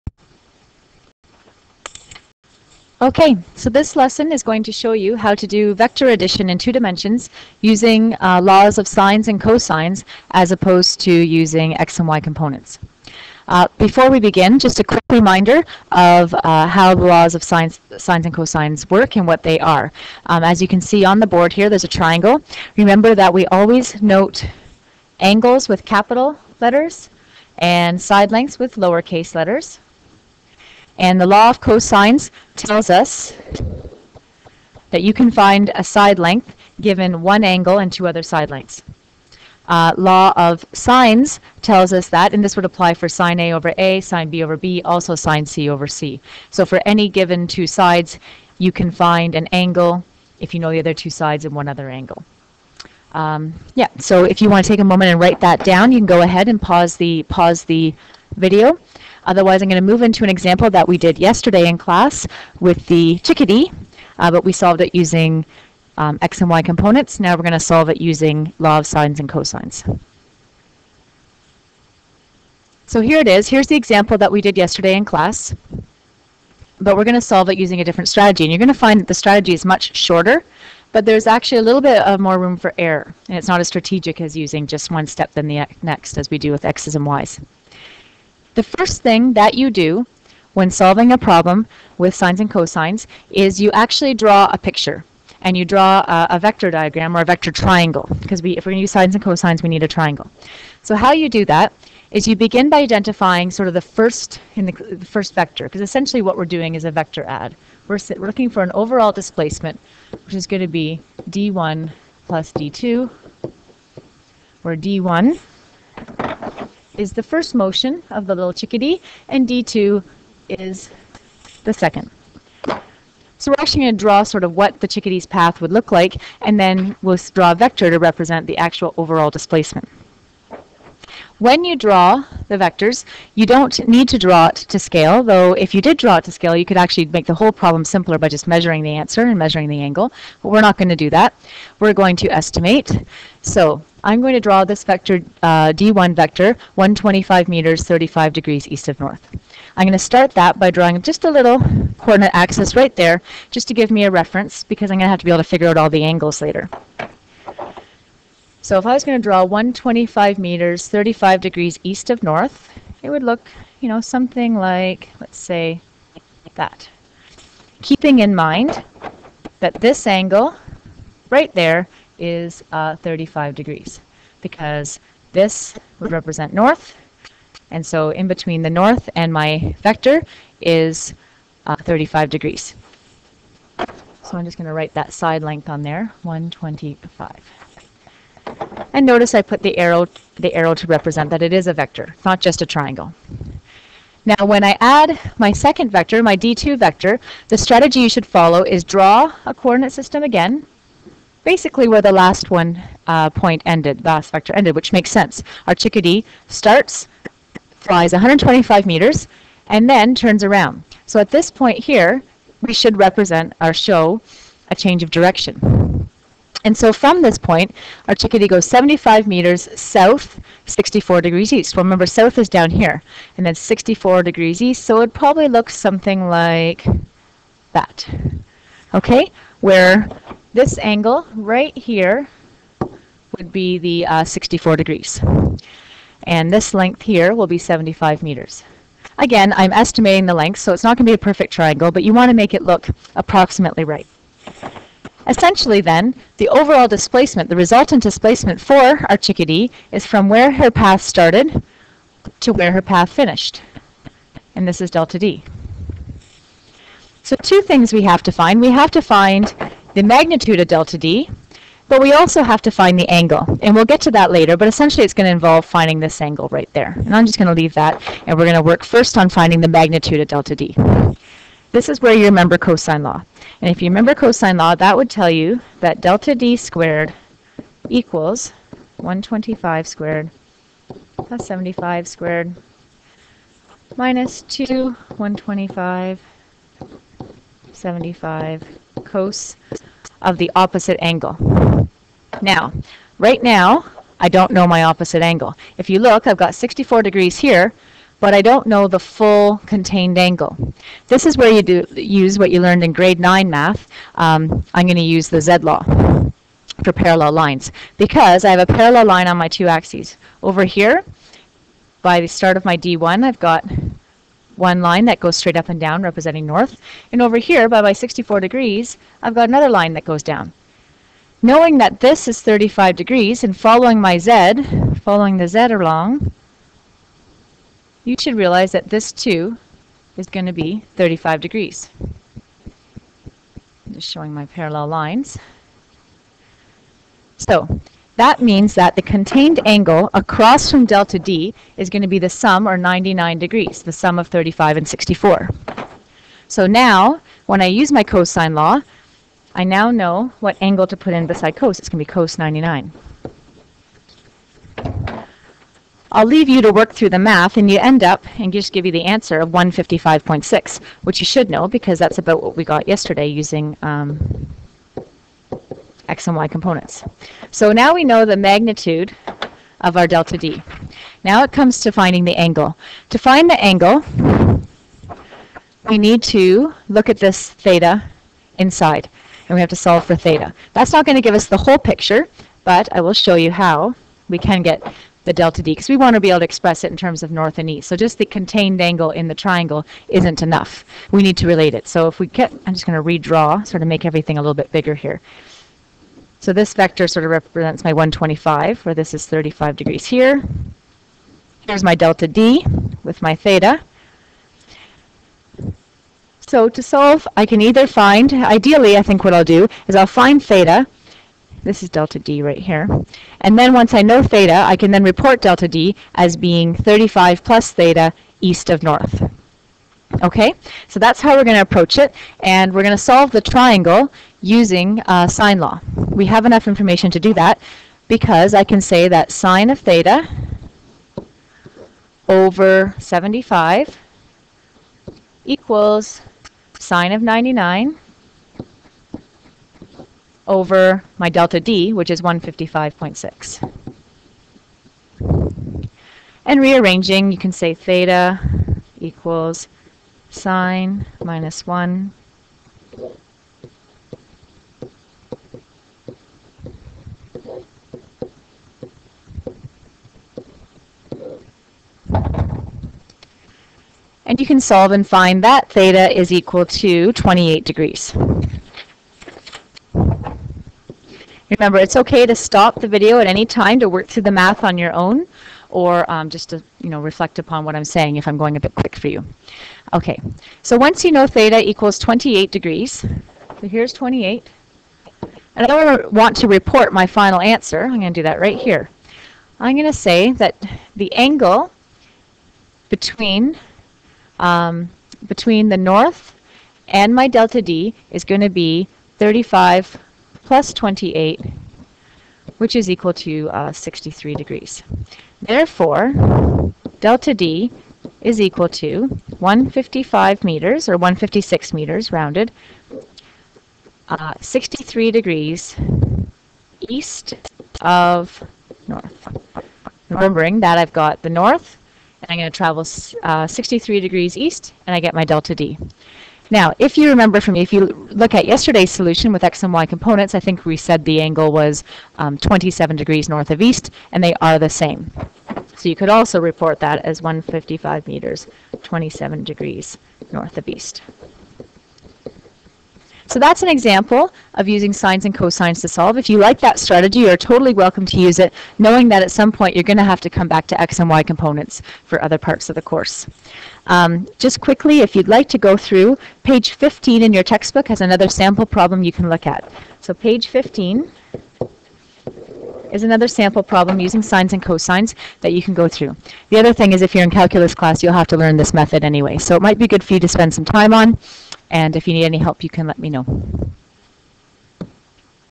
SPH4U Vector Addition Using Trig
Short lesson with example on adding vectors using the laws of sines and cosines for SPH4U Students at Stratford Northwestern Secondary School